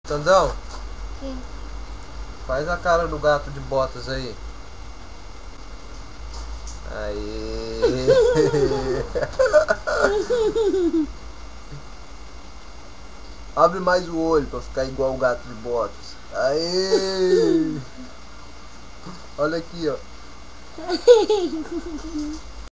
Imitando o Gato de botas
Olha o sanderson imitando o gato de botas kkkkkkk
é claro que isso é um efeito de video,mas ficou engraçado.